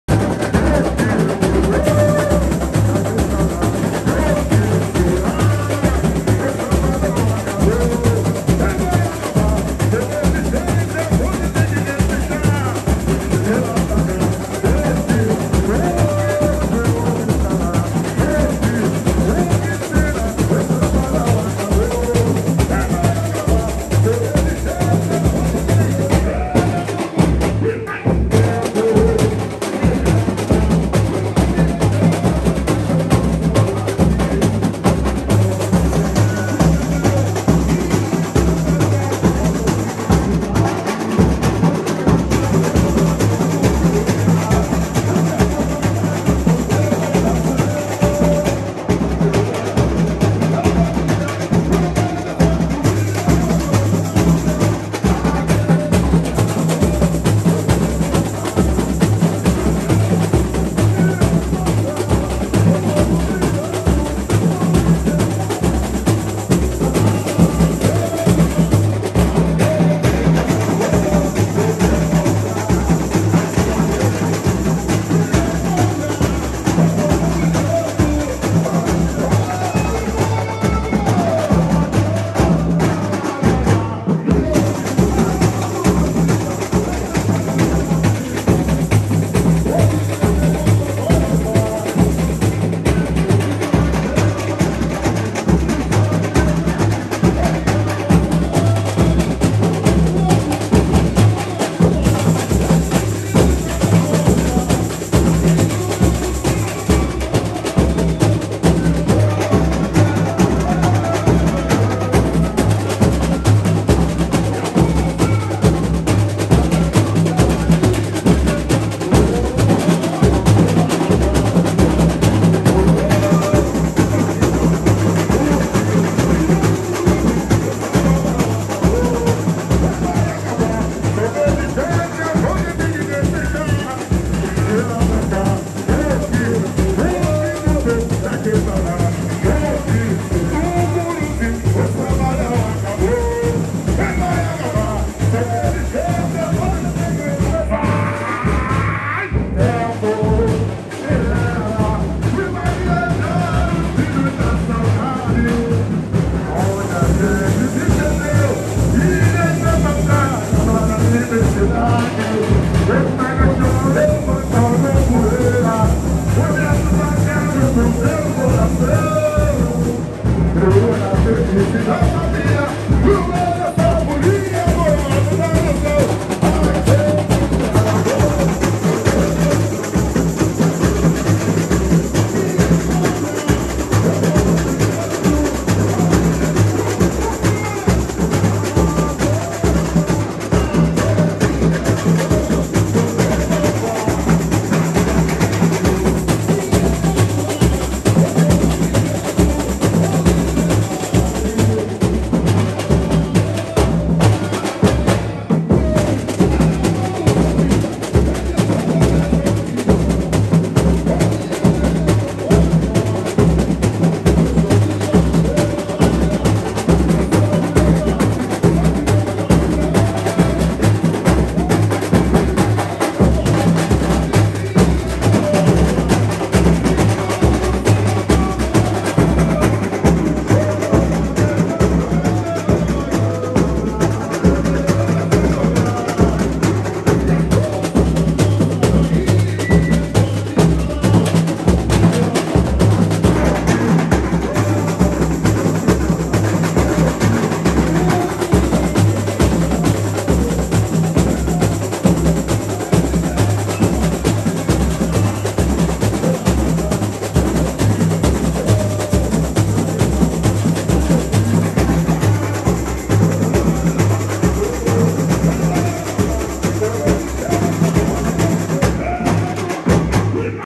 Bateria do Zaca - Segundo Ensaio de Quadra Rumo ao Carnaval 2022 - Acadêmicos do Tucuruvi
Segundo Ensaio da Bateria do Zaca, comandado pelo Mestre Serginho, Rumo ao Carnaval 2022 de São Paulo.

Ensaio realizado na quinta-feira, dia 07 de Outubro de 2021.

Imagem @renatocipriano